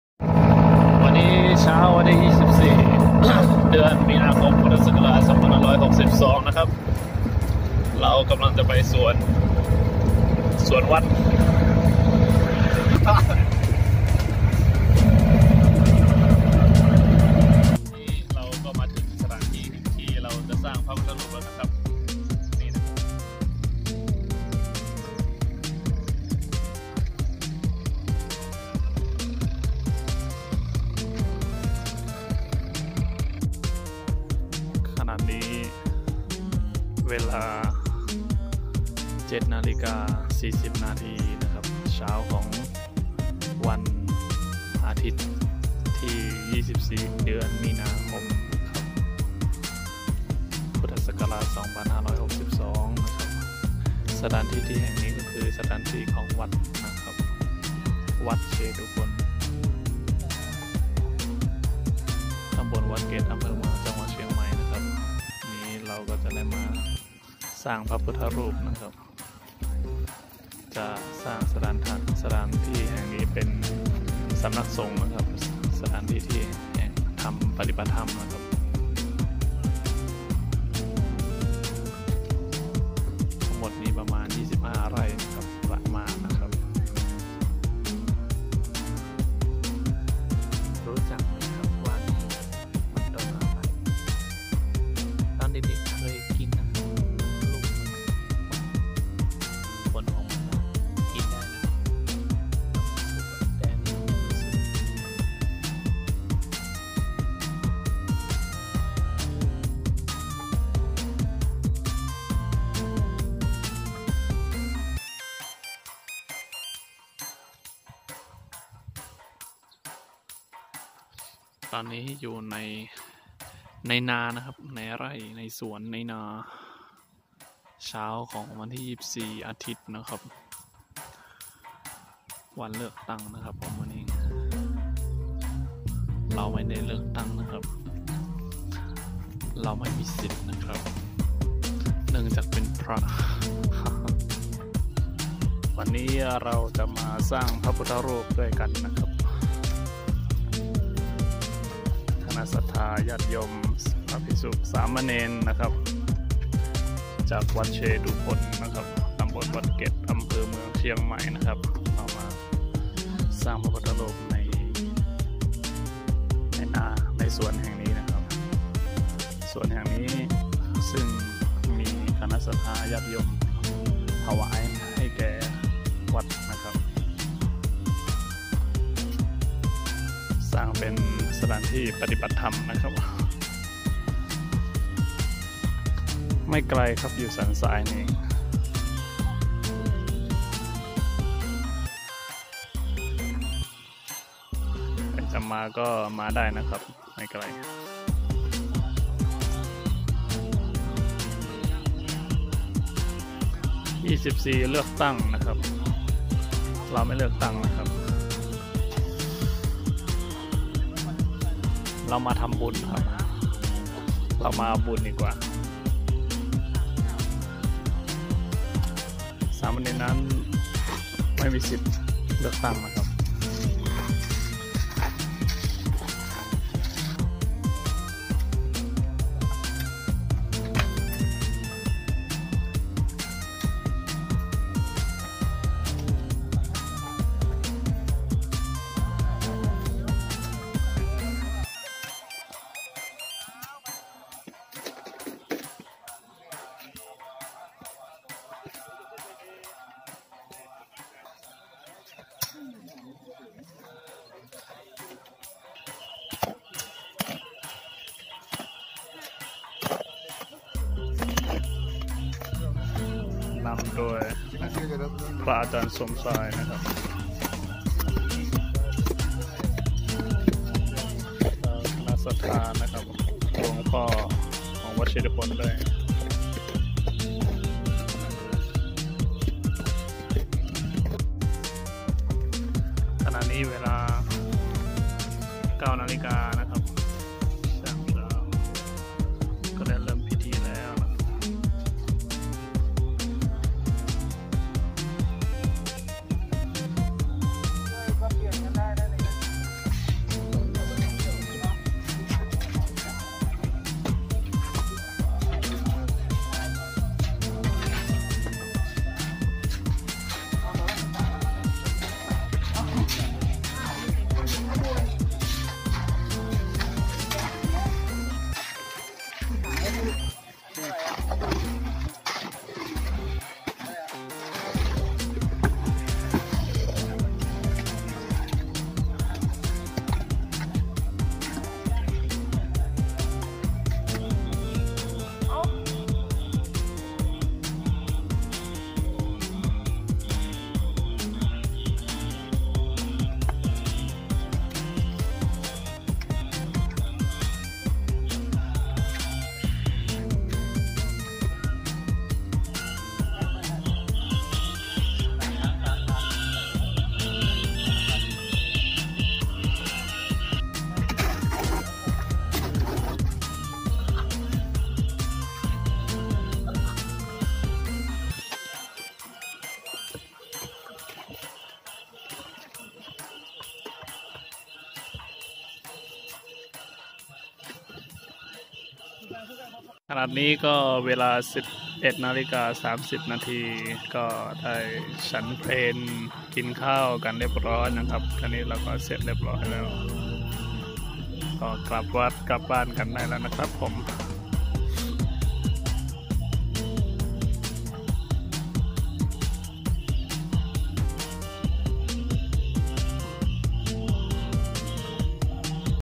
หล่อพระสร้างพระพุทธรูป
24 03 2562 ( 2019 ) หล่อพระสร้างพระพุทธรูป คณะศรัทธาญาติโยมและ พระภิกษุสามเณรนำโดยหลวงพ่อ วัดเชตุพน  ได้ไป สร้างองค์พระ การสร้างองค์พระพุทธรูปนั้นเป็นบุญมหาศาลอันยิ่งใหญ่ ฉะนั้น ถ้ามีโอกาส ก็ให้สร้างกันนะครับ หรือถ้าได้มีโอกาสไปร่วมบุญก็ไปร่วมสร้างด้วยกันนะครับก็จะได้อานิสงส์บุญด้วยกัน
ก็ขออนุโมทนาบุญกับคณะศรัทธาญาติโยมและพระภิกษุสามเณรทุกคนทุกรูปทุกท่านด้วยนะครับก็ขอให้มีขอให้อานิสงส์บุญแห่งการหล่อพระในครั้งนี้จงบันดาลให้กับทุกท่านมีแต่สุขภาพพลานามัยแข็งแรงแข็งแรงอายุมั่นขวัญยืนนะครับเกิดไปตายไปชาติหน้าเกิดใหม่ก็ขอให้สวยหล่อผิวพรรณผ่องใสขอให้มีสมองเฉลียวฉลาดเกิดในฐานะผู้ดีเราก็เป็นคนดีของสังคม และขอให้มีเงินทองร่ำรวยร่ำรวยนะครับและขอให้เป็นนักบุญ และสืบทอดศาสนาพระพุทธศาสนาด้วยนะครับ ขอให้ทุกท่านหวังสิ่งใดปรารถนาสิ่งใดก็ขอให้สมดังปรารถนาทุกประการด้วยเทอญ อายุ วัณโณ สุขัง พลัง